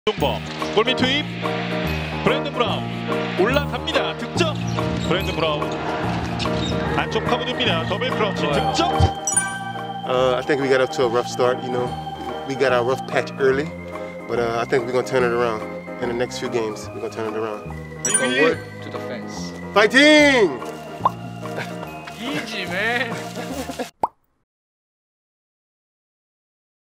KT소닉붐에 합류한 브랜든 브라운!! 그 소감을 한마디로 표현하면~?
브랜든 브라운의 이번 시즌은 KT소닉붐과 함께!!
시즌에 임하는 자세를 한 마디로 담아보았습니다
직접 확인하시죠!!